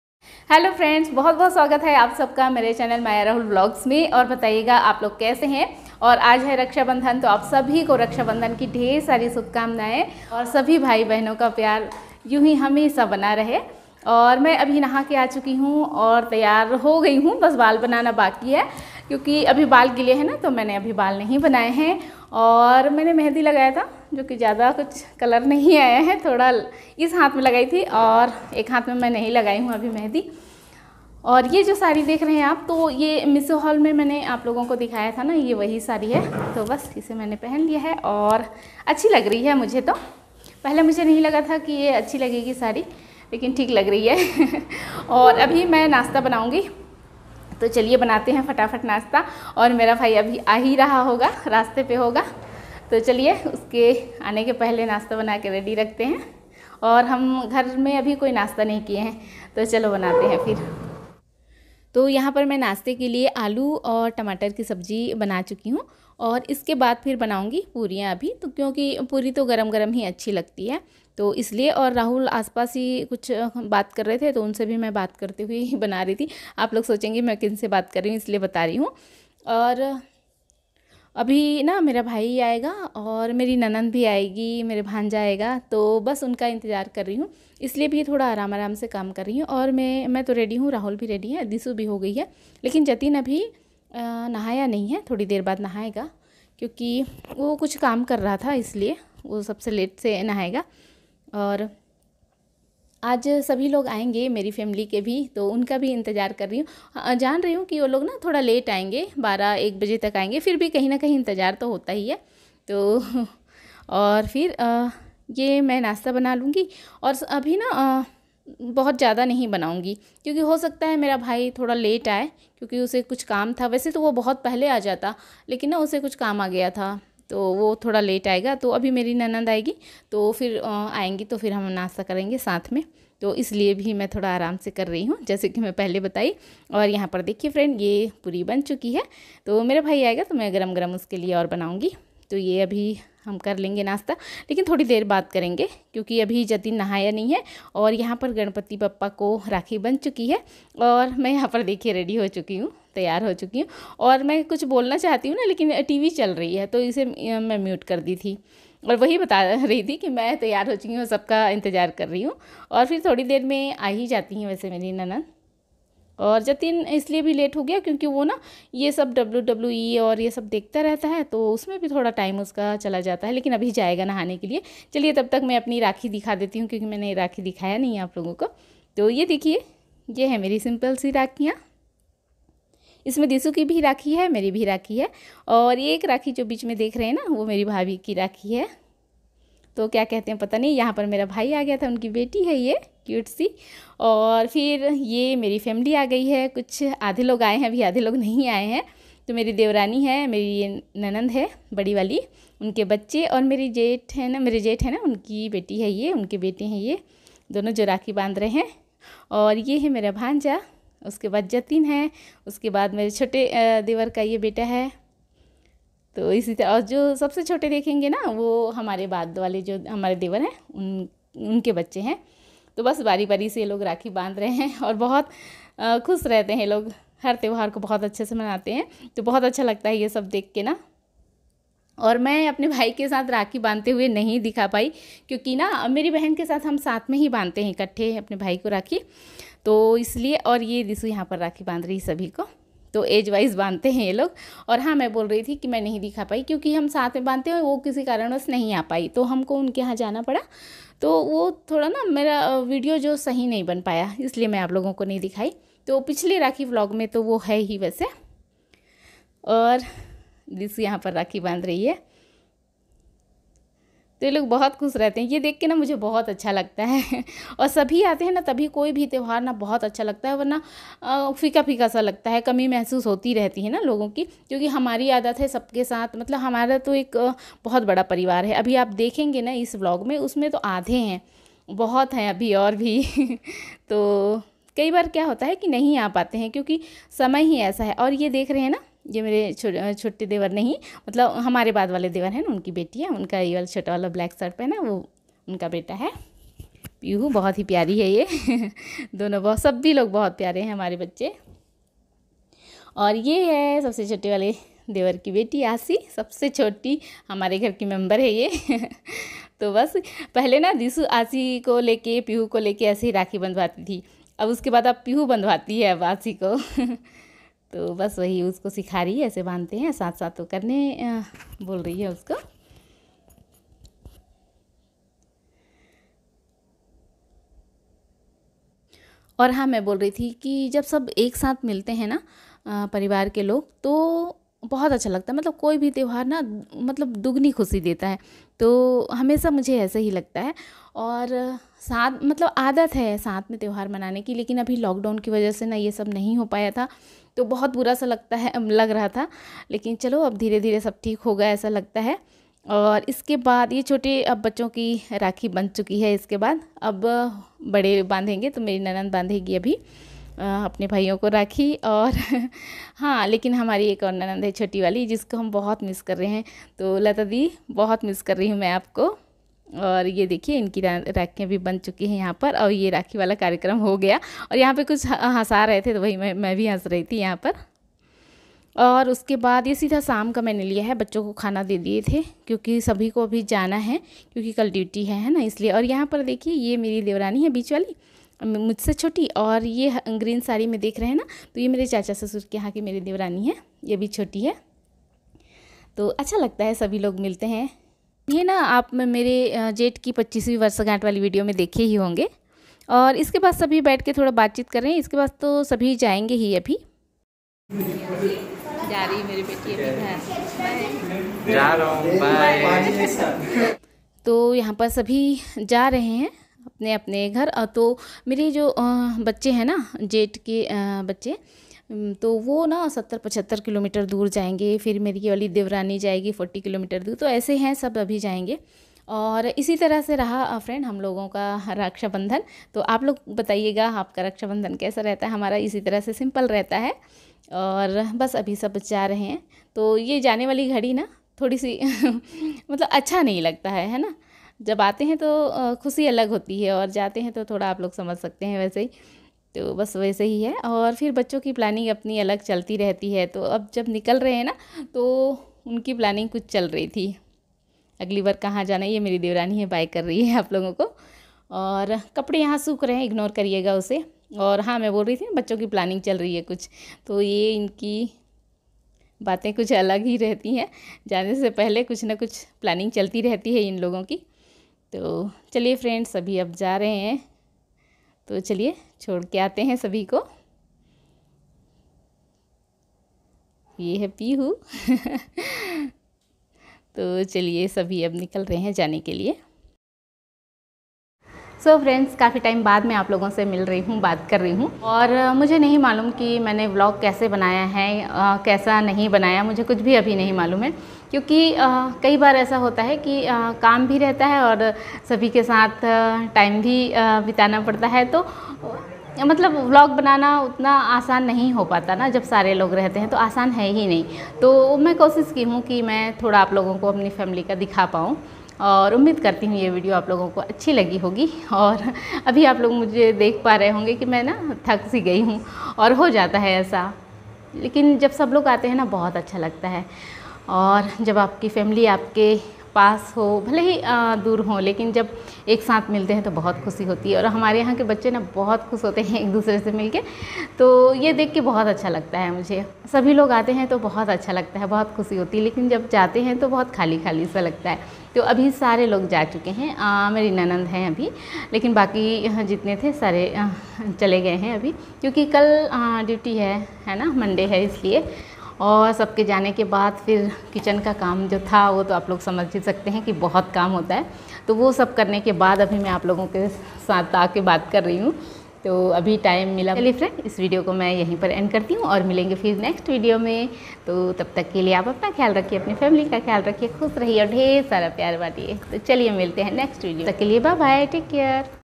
|| हम सब ने ऐसा मनाया रक्षाबंधन का त्योहार 🤗 || Maya Rahul Vlogs ||
Happy rakshabandhan friends.

Aaj es video me hamne rakshabandhan ka त्योहार kaise मनाया apni family ke sath wahi es video me maine dikhane ki koshish ki h . I hope aap sabko ye video pasand aayega pasand aaye to Please video ko like aur channel ko subscribe jarur kijiyega 🙏

Happy rakshabandhan
Aalu puri breakfast